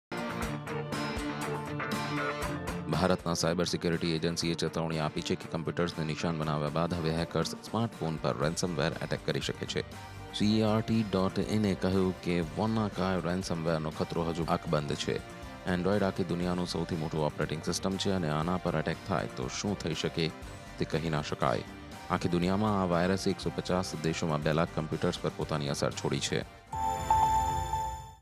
Ransomware attack isn't over, Android smartphones could be next target
Ransomware infections on computers have hundreds of thousands of computers around the world were infected this week by a massive ransomware attack. but soon next target could be the